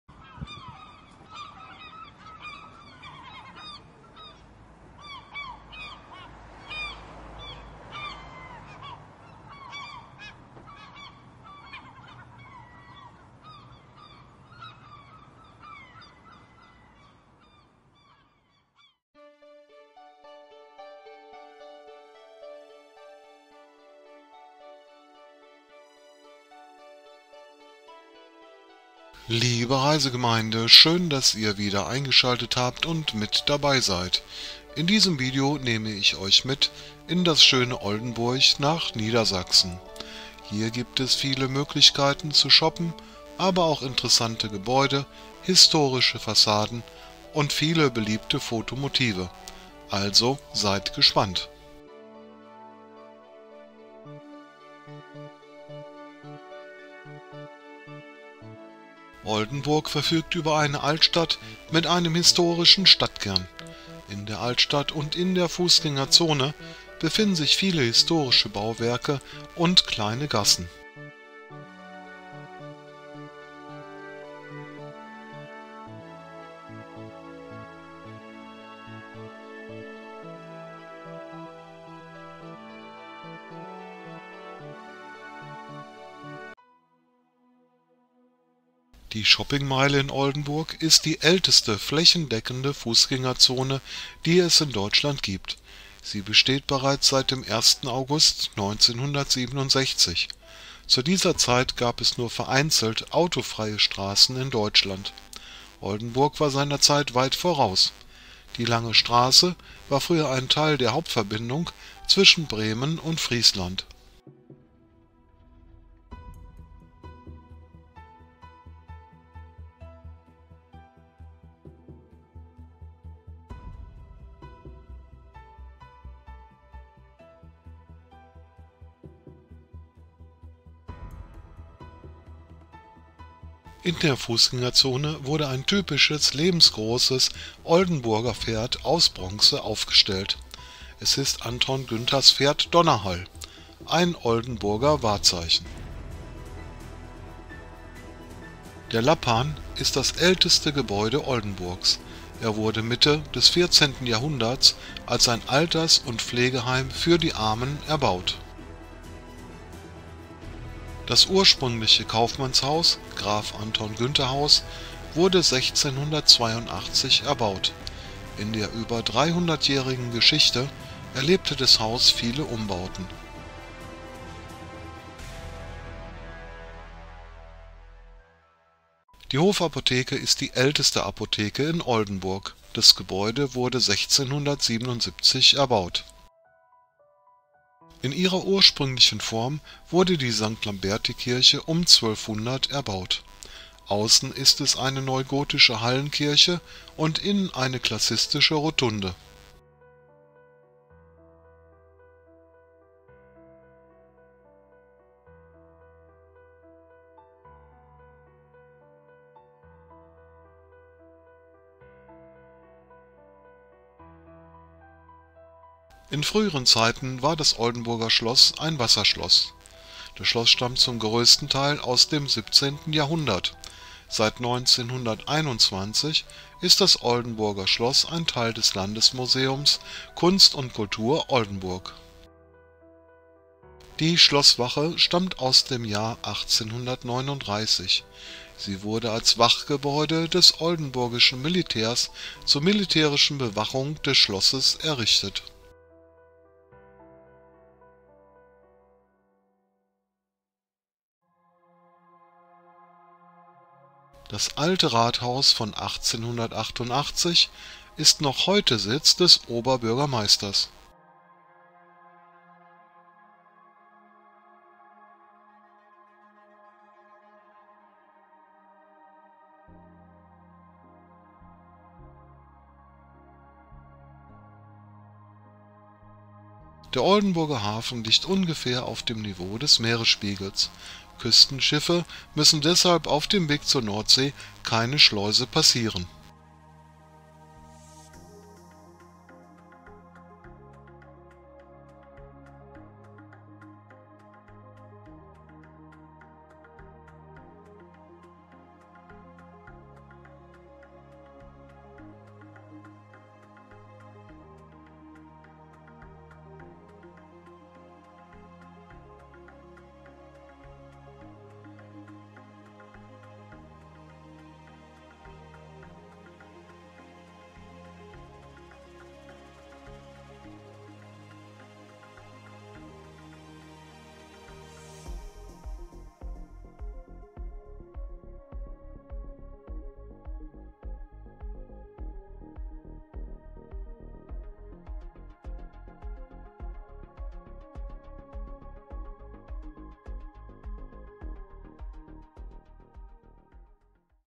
Oldenburg in Niedersachsen | Ausflugsziele
Liebe Reisegemeinde! Schön, dass ihr wieder eingeschaltet habt und mit dabei seid. In diesem Video nehme ich euch mit in das schöne Oldenburg in Niedersachsen. In Oldenburg gibt es viele Möglichkeiten zu shoppen, aber auch interessante Gebäude, Fassaden und beliebte Fotomotive. Oldenburgs Schloss, Parks und Gärten sind sehr sehenswert!

VIDEOGRAFIE / FOTOGRAFIE / SCHNITT
Claus Wolfgramm

MUSIK
Musik von Ronald Kah

MUSIK LIZENZ
Diese Musik ist lizenziert unter einer Creative Commons Namensnennung 4.0 International Lizenz.

2-- Du wünschst Dir ein spezielles Niederrhein Video? Dann schreibe in die Kommentare.